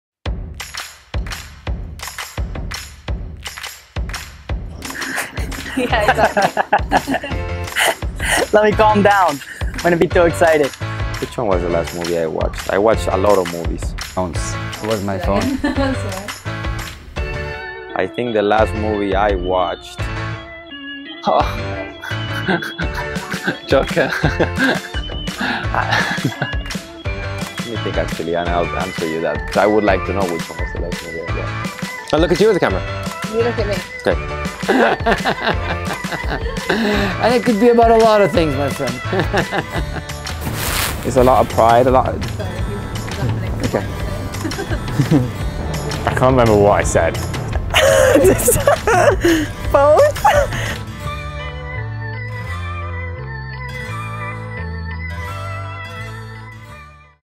2019 - Bloopers of the year!
This year we made more than 100 interviews of riders, grooms, owners, organisers, etc.

But not all of them went as planned...

Kent Farrington, Olivier Philippaerts, Daniel Bluman, Steve Guerdat, Cormac Kenny, Sean Vard, Eric Lamaze, Janika Sprunger, Harry Charles, Sean Lynch, you are officially on the best bloopers of 2019!